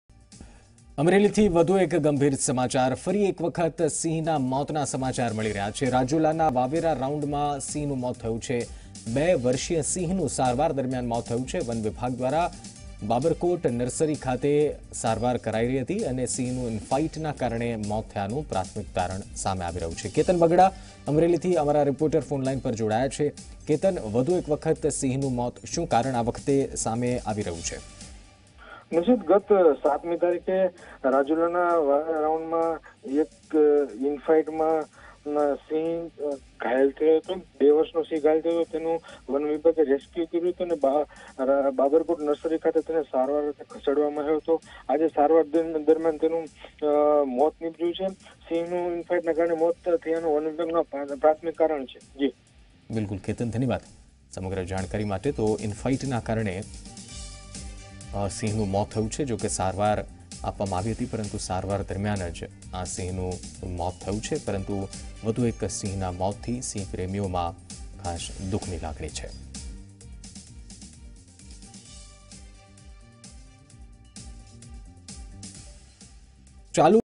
Death of lion during treatment at Vavera round rajula Amreli | Zee 24 Kalak
Death of lion during treatment at Vavera round rajula Amreli